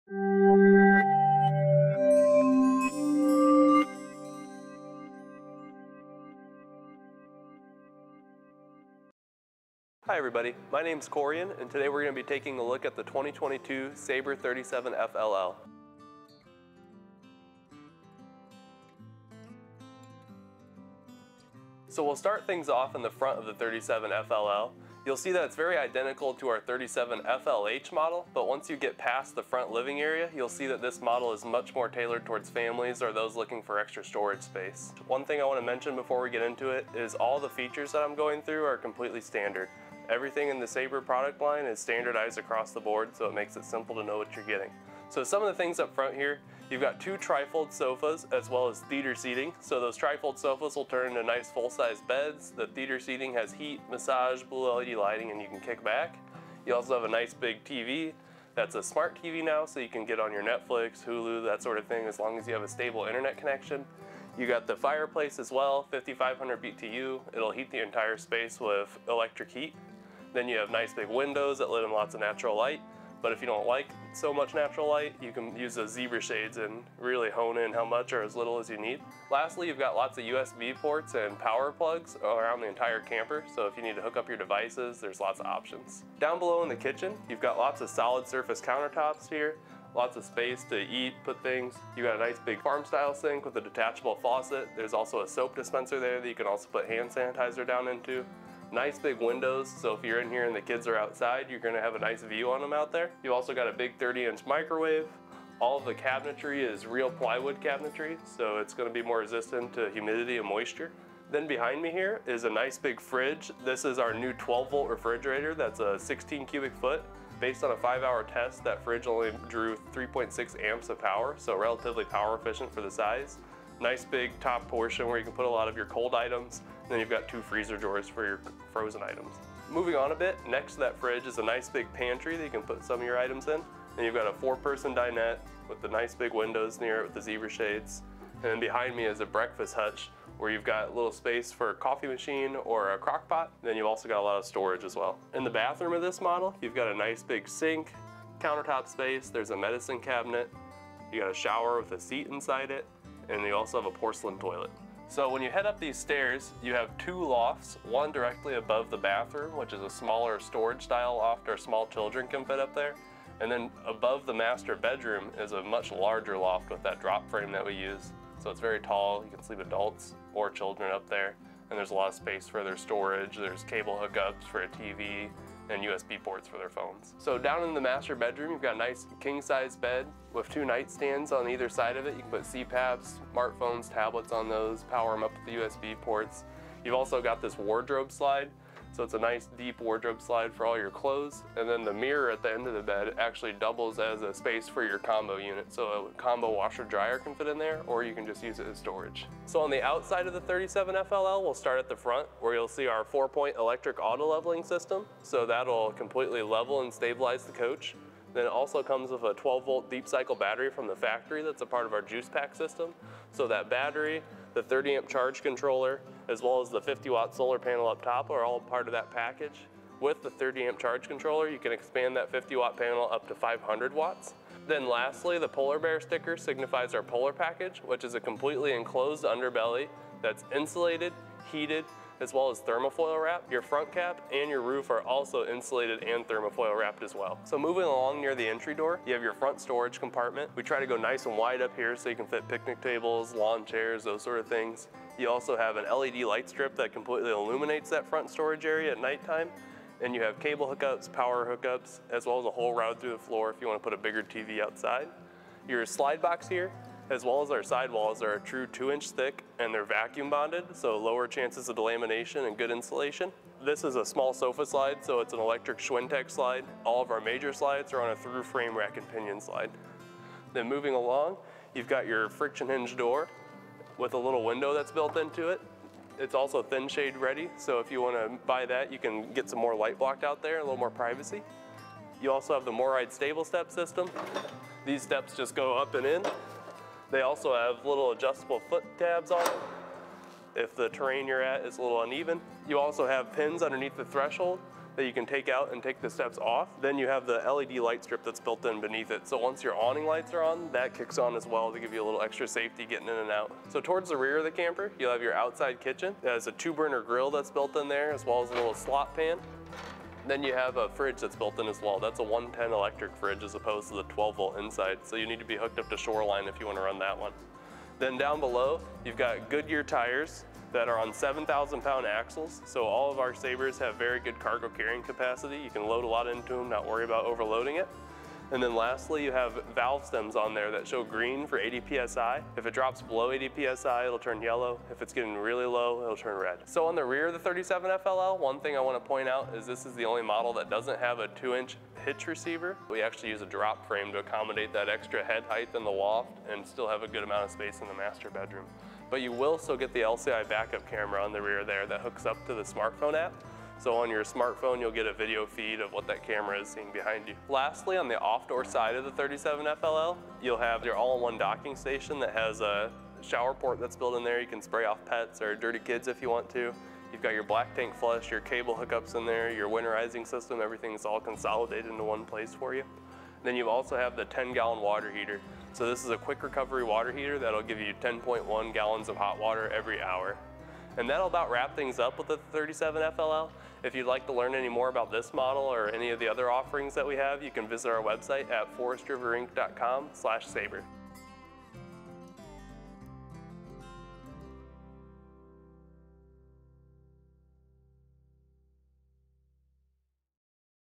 Front Living Room + Bunks! - 2022 Sabre 37FLL Walkthrough
Get camp ready and unplug in the 2022 Forest River Sabre 37FLL. This Fifth Wheel takes comfort to a whole new level and comfortably sleeps 6. 2 sofa bed(s) and 1 queen bed helps your family enjoy nighttime dream adventures too. And contains a Bunkhouse. And a master bedroom conveniently located in the Rear. This Sabre Fifth Wheel has a lot to offer the RV adventurer, like a Center kitchen with Chairs, a Front living area, and a Center bathroom. The Sabre 37FLL Fifth Wheel makes on-the-road living easy with features like a stove with 3 oven burners, a Full-Size refrigerator, 1 TV, Standard Fireplace, and more. This Sabre 37FLL has a payload capacity of 2972 lbs. It also offers 1 Black Water Holding Tank(s), 1 Fresh Water Holding Tank(s), and 2 Gray Water Holding Tanks. This Sabre has 1 door(s), 1 awning(s), 4 slideout(s).